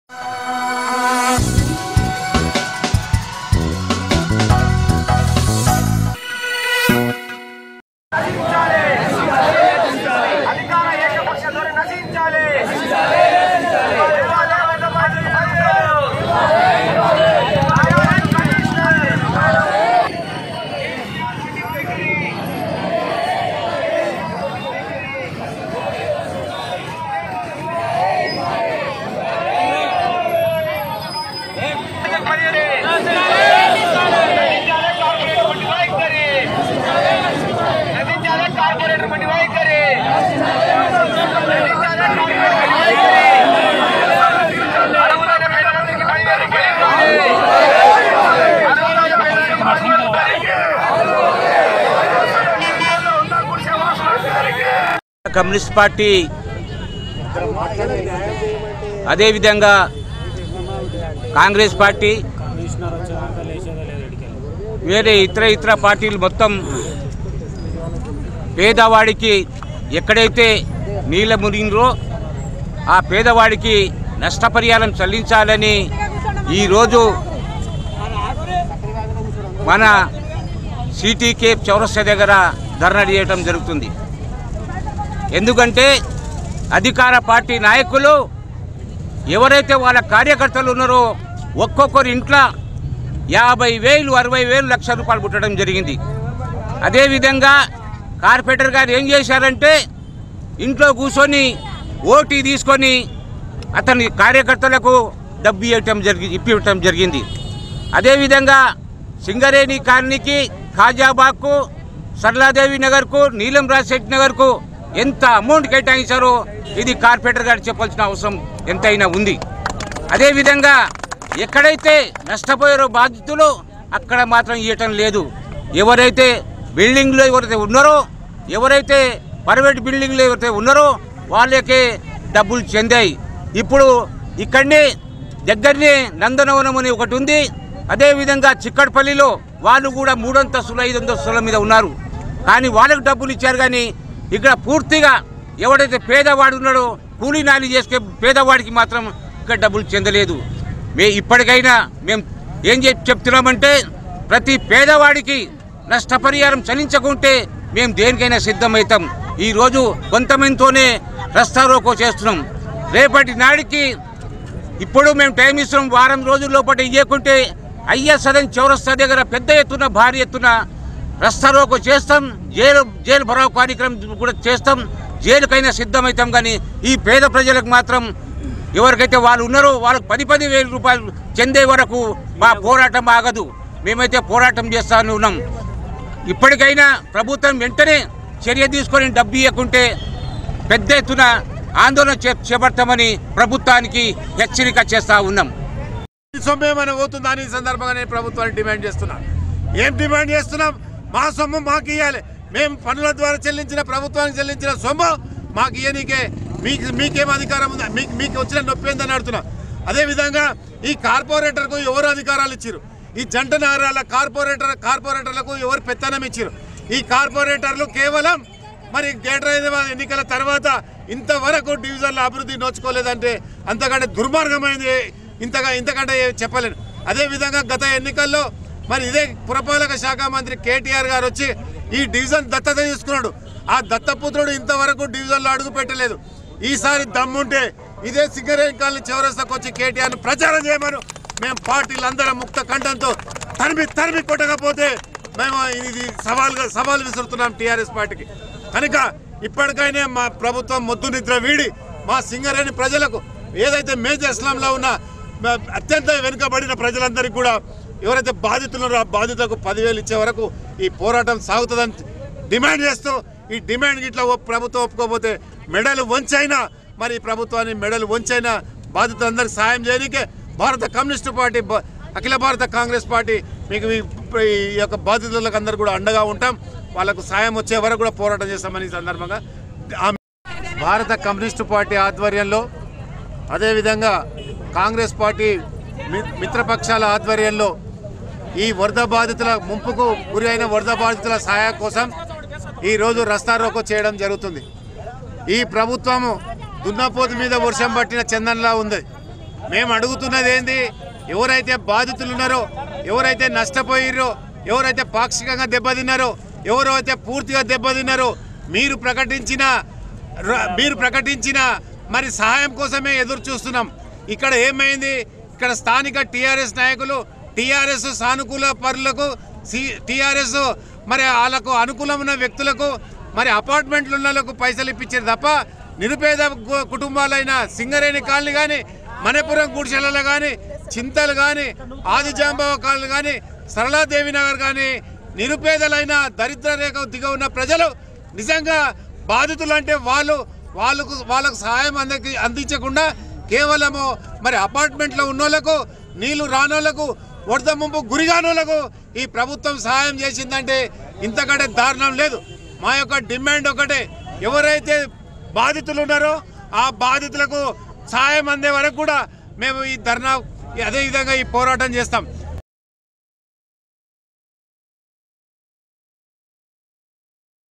CPI, CPM, TDP & Congress Protest Over Unfair Disbursement of Flood Relief in Hyderabad | Overseas
CPI, CPM, TDP & Congress Protest Over Unfair Disbursement of Flood Relief in Hyderabad